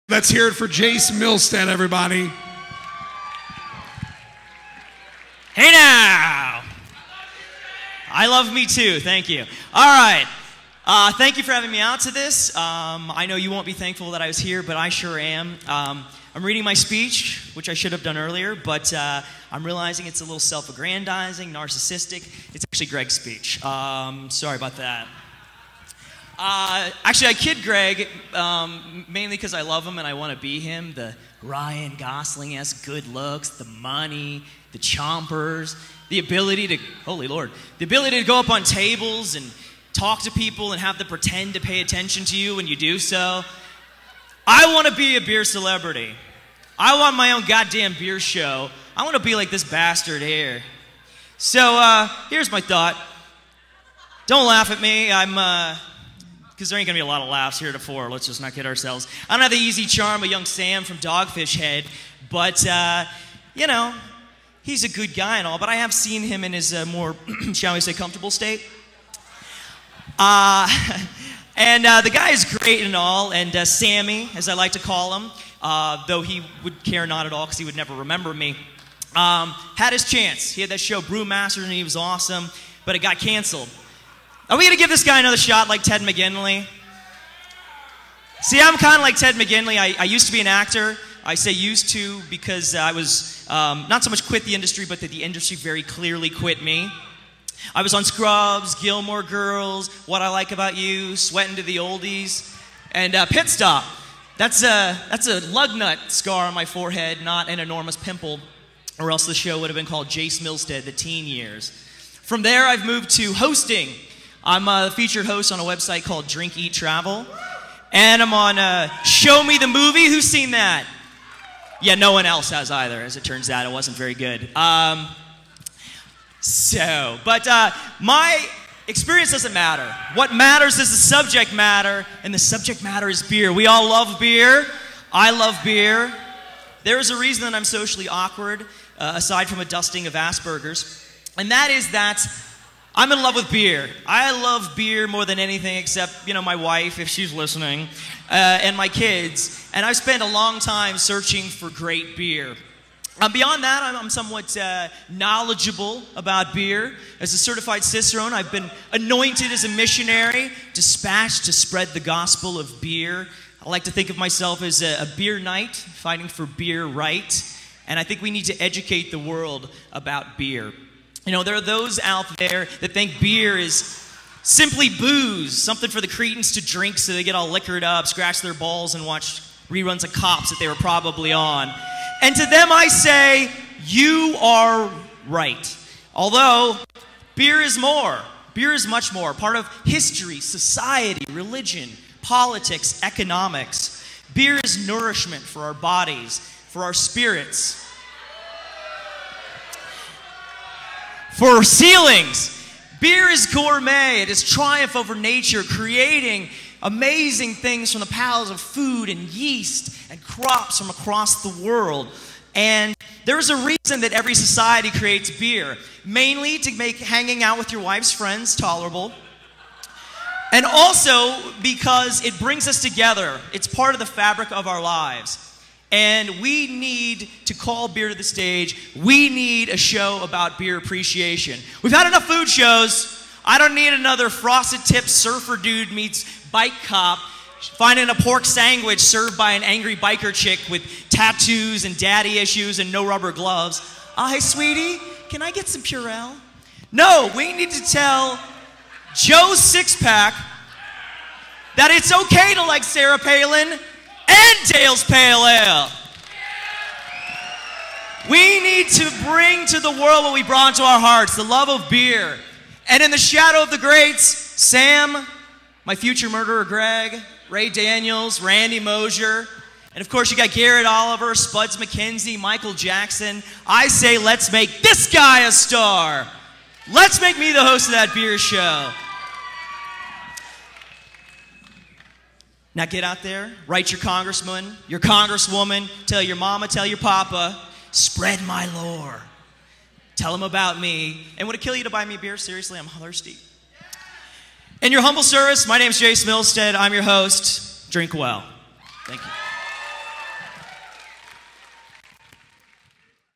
ID10: Jace Milstead
Jace Milstead presents at Ignite Denver 10 September 27, 2011 at the Sherman Street Event Center in Denver, Colorado